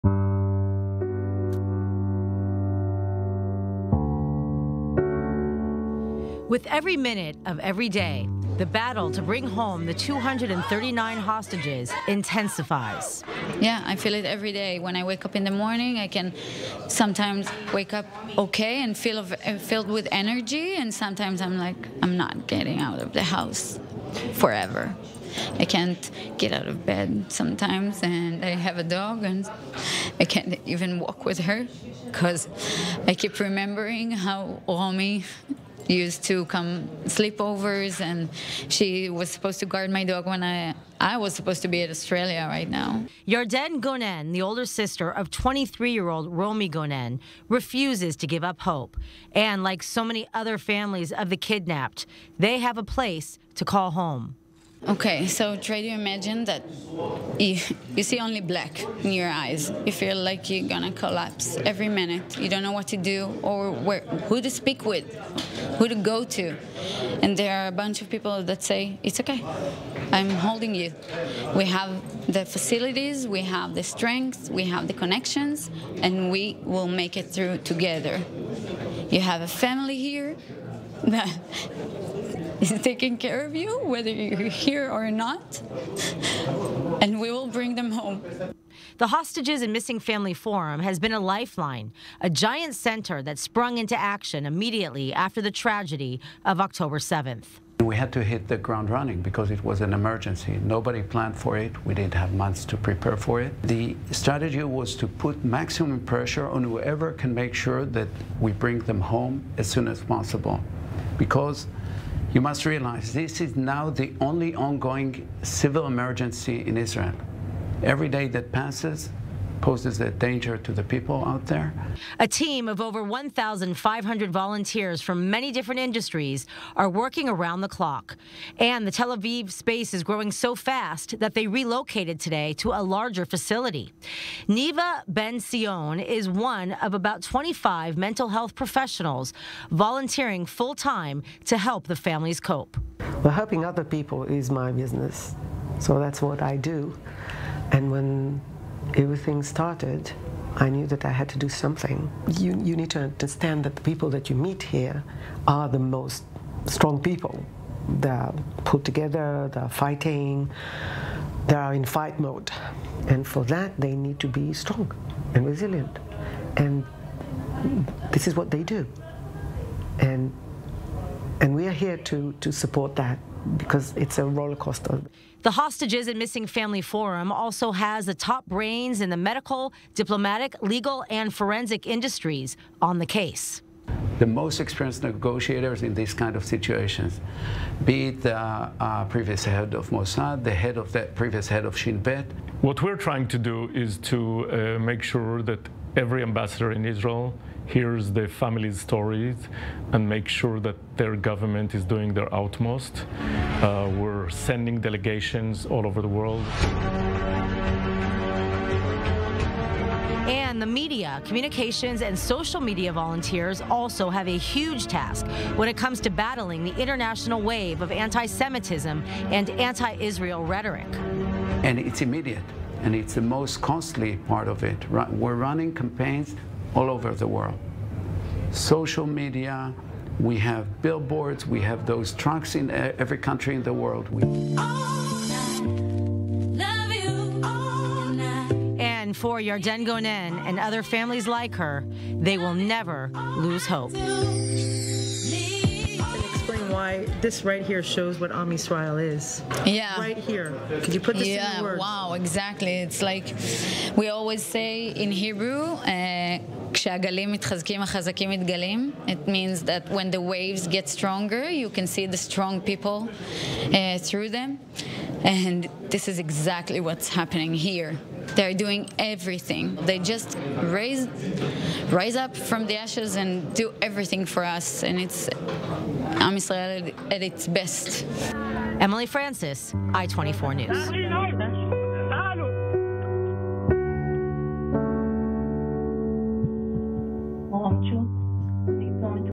HOSTAGES AND MISSING FAMILY FORUM-BRING US BACK
A report inside the HQ of the 1500 Volunteer operation helping the families of the Hostages in Gaza and also working tirelessly to bring them home.